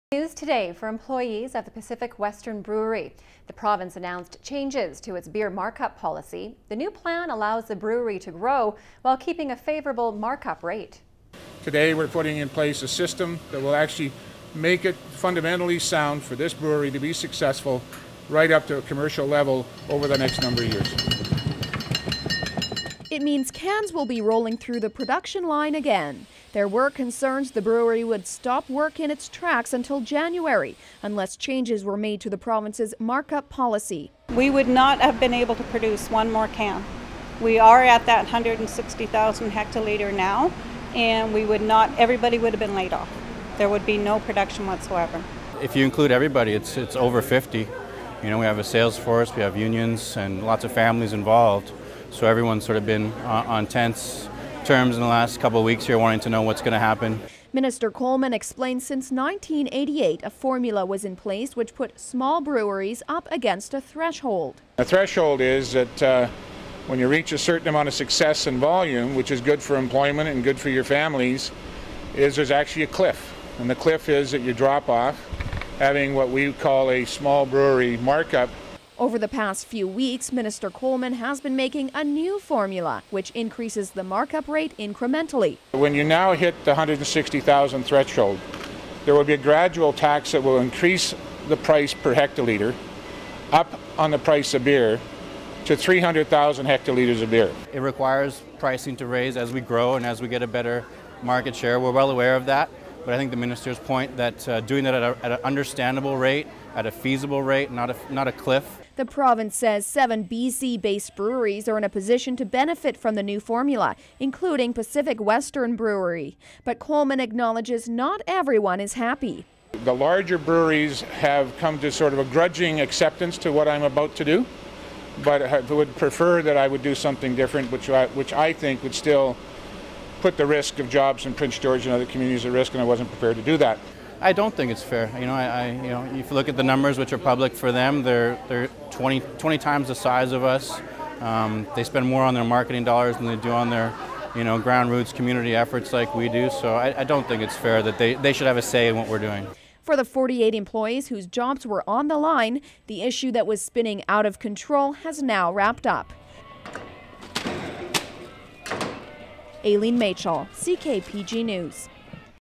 Mark-Up Helps Pacific Western Brew's Bottom Line
Some good news today for employees of the Pacific Western Brewery. The province announced changes to its beer mark-up policy. The new plan allows the brewery to grow, while keeping a favourable mark-up rate.